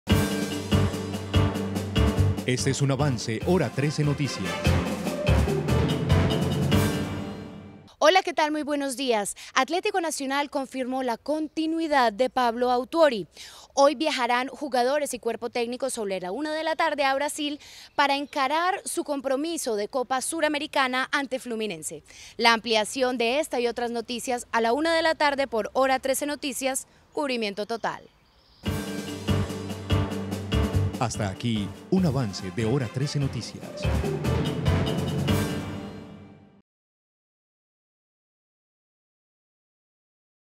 Atlético Nacional confirmó la continuidad de Paulo Autuori
Atlético Nacional confirmó la continuidad de Paulo Autuori; hoy viajarán jugadores y cuerpo técnico a Brasil.
Detalles de esta y otras noticias a la Hora 13